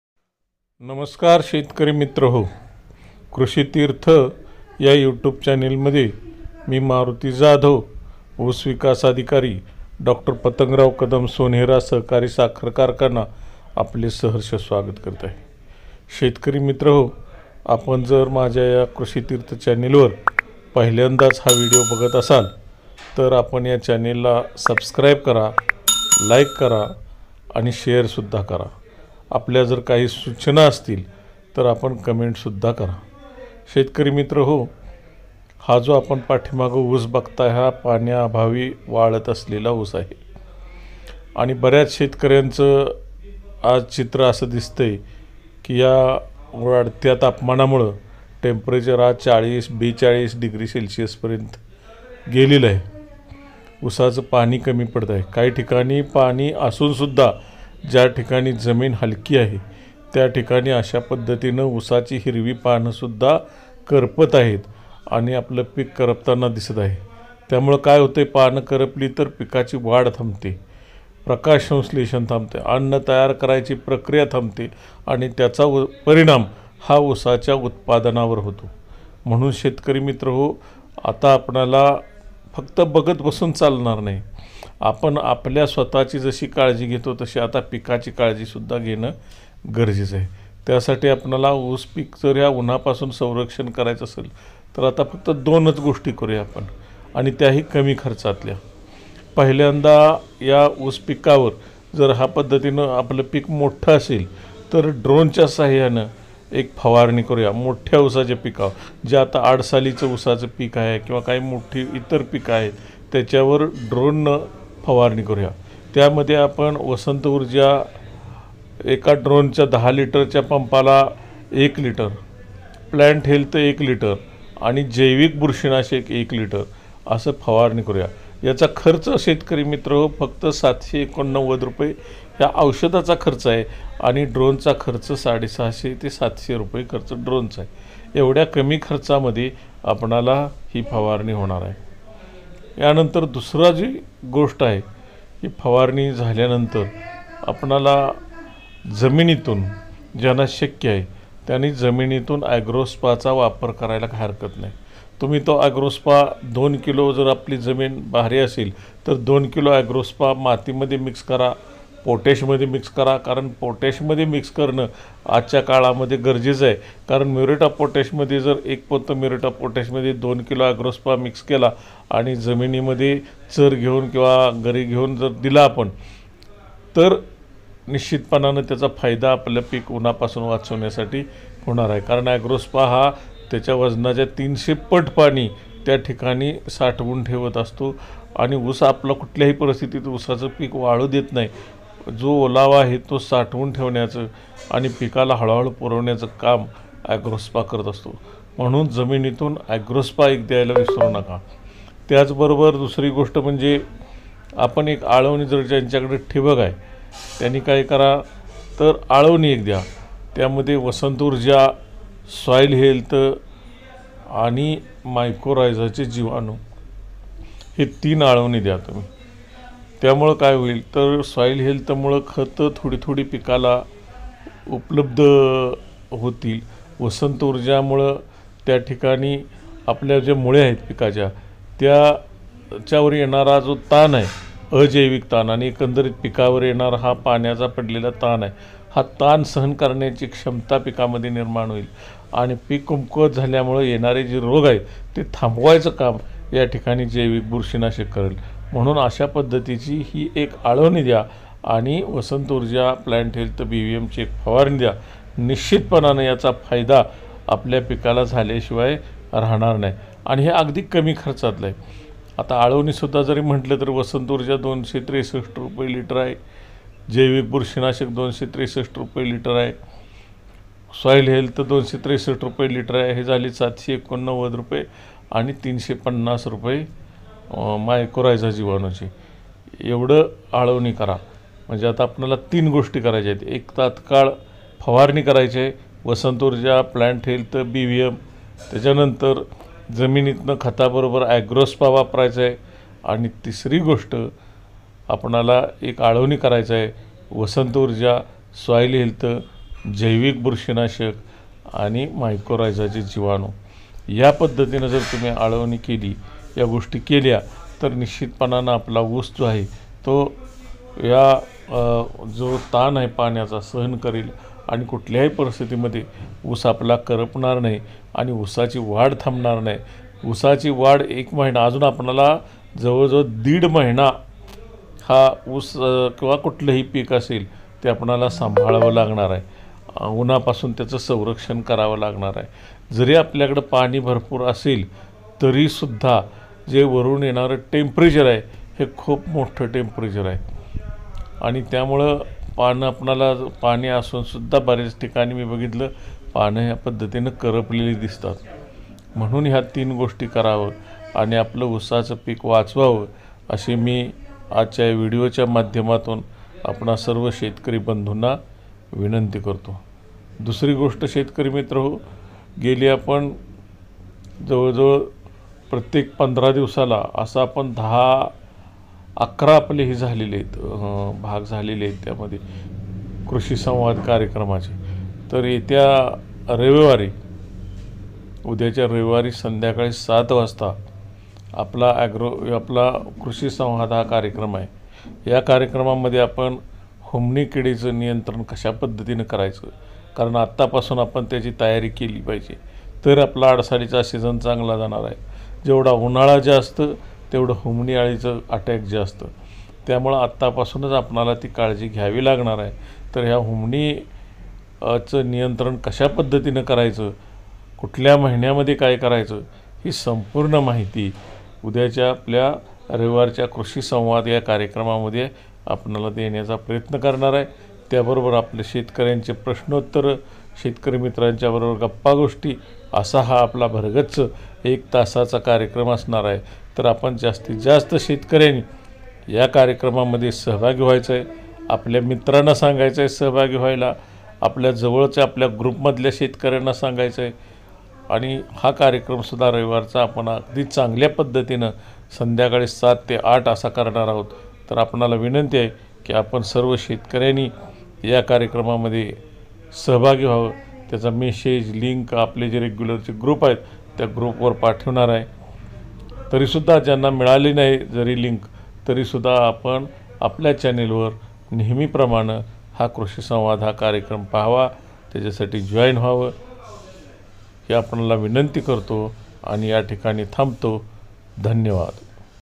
उन्हापासून ऊस पिकाचे संरक्षण करण्यासाठी करा ही एक फवारणी व एक आळवणी।100%रिझल्ट।@KrushiTirth
उन्हापासून ऊस पिकाचे संरक्षण करण्यासाठी करा ही एक फवारणी व एक आळवणी।100%रिझल्ट।‎@Krushi Tirth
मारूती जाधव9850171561